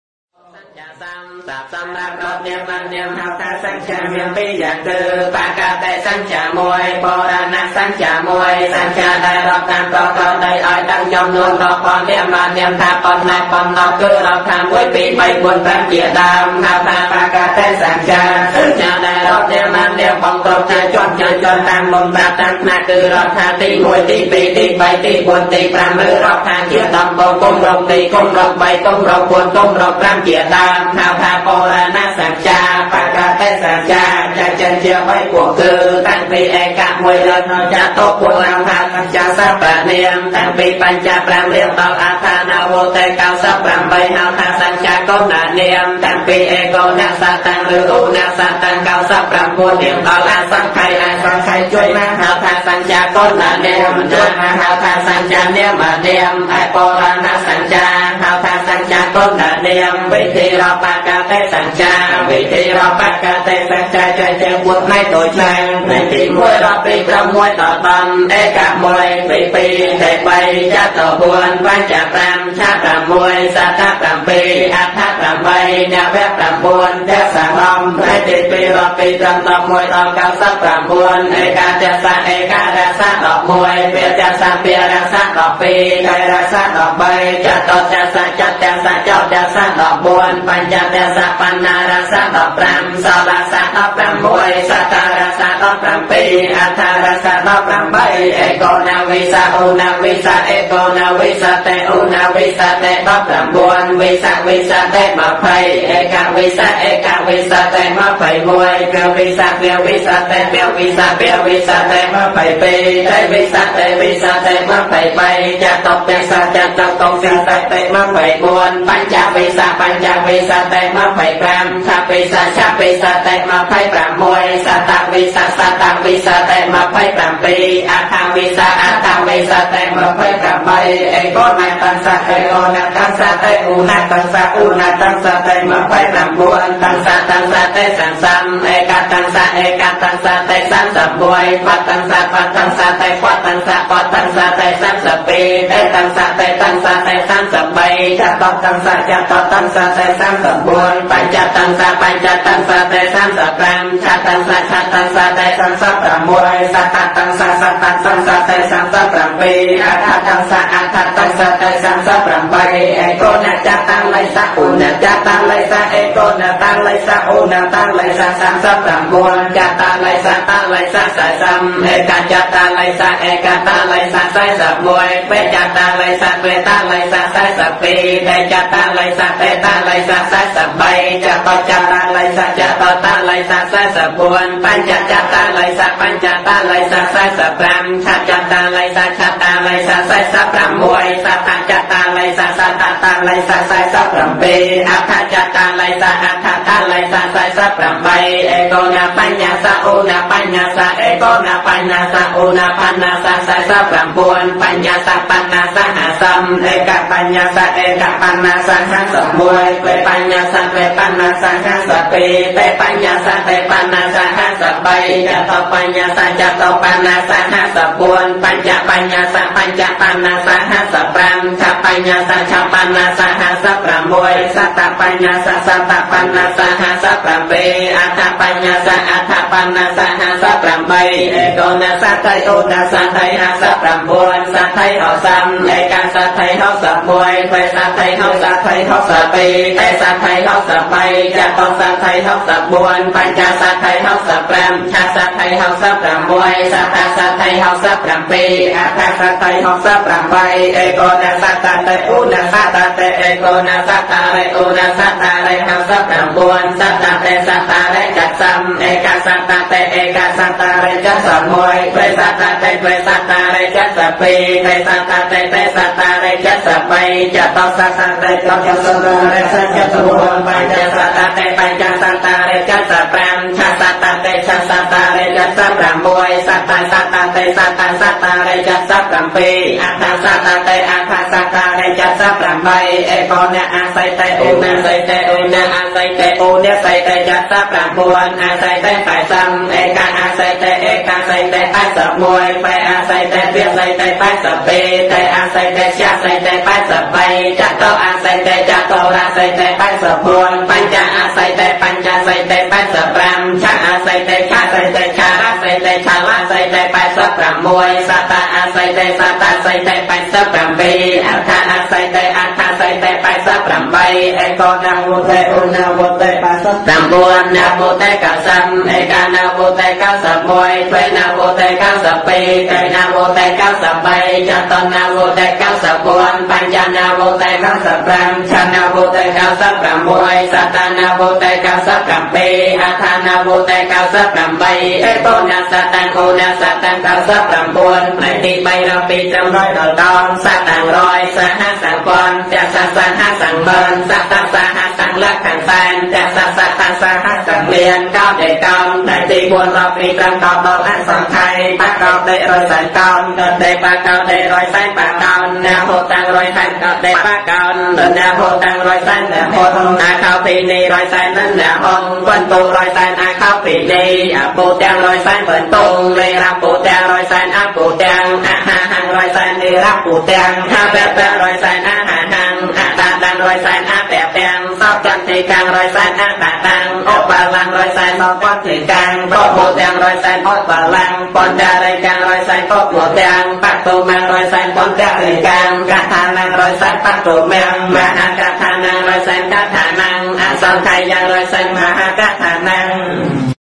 របៀបរៀនបាលីវេយ្យករណ៍/នាមនិទ្ទេសទី២/សង្យាសព្ទ/pali grammar 07
រៀនភាសាបាលីវេយ្យករណ៍ Learning Grammar Pali(សៀវភៅ សម្តេច ជួន ណាត, សៀវភៅ បា្រក់ ឃុន,សៀវភៅ សេក នាង )
សូត្រដោយ៖ ព្រះសង្ឃខ្មែរ
✅សូមឲ្យព្រះពុទ្ធសាសនារុងរឿងចម្រើនយូរអង្វែងរហូតជានិរន្តរ៍
✅ការស្តាប់ព្រះធម៍ដោយល្អរមែនបាននូវប្រទីបគឺ បញ្ញា
✅សូមអរគុណសម្រាប់ការគាំទ្រនូវឆានែលព្រះធម៌នេះ!